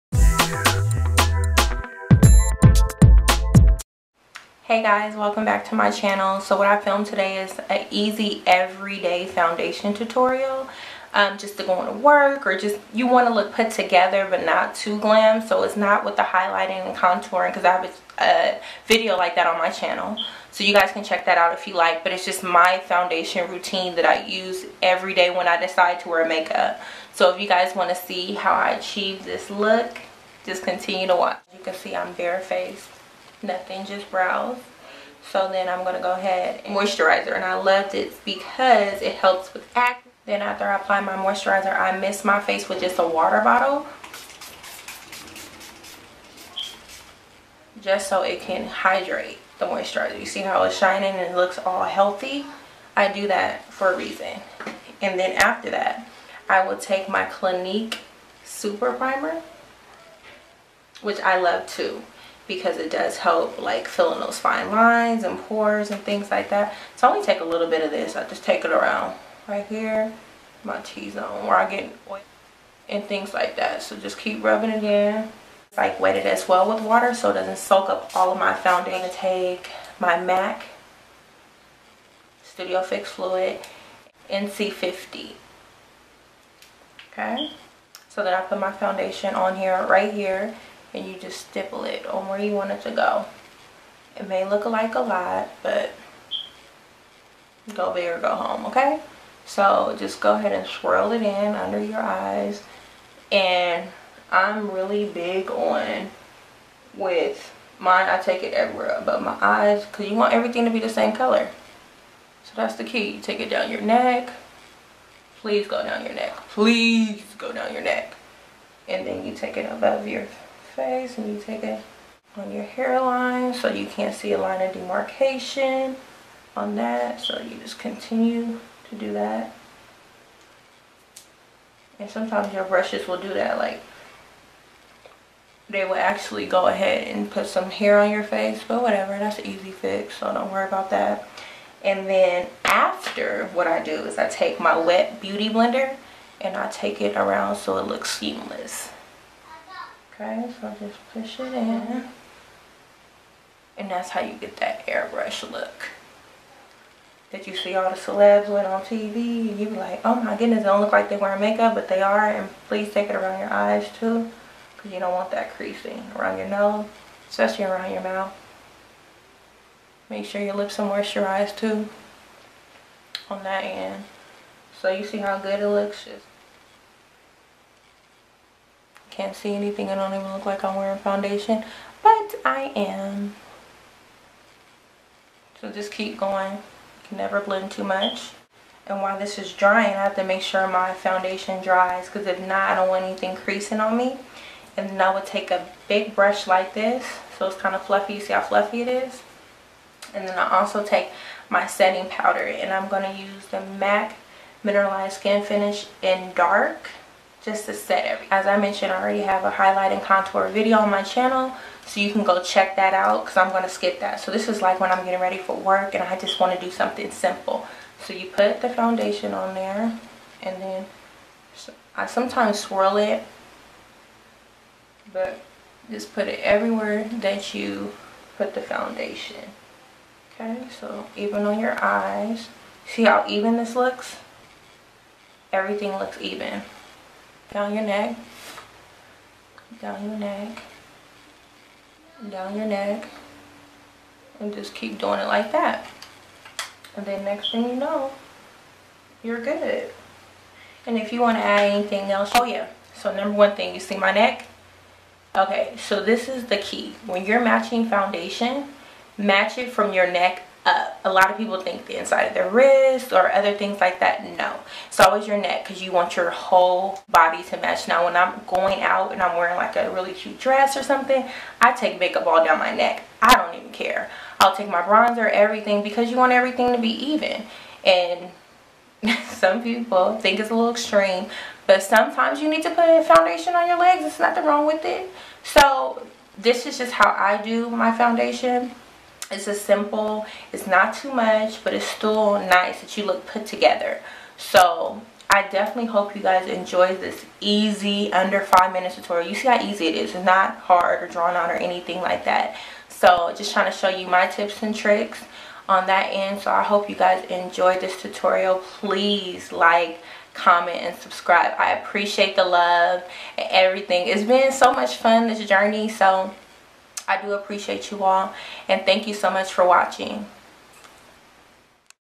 5 MINUTE EVERYDAY FLAWLESS FOUNDATION TUTORIAL | THEMSAPPLE07
Hello gorgeous,

I filmed this everyday foundation tutorial that will be perfect for school, work or  any everyday foundation where your skin can still look flawless but it will save you much time when you are in a hurry.

-Foundation- Mac studio fix fluid in NC 50
-Applied with a stippling foundation brush and after I used my beauty blender to blend out for that flawless airbrush foundation application.

xoxoxo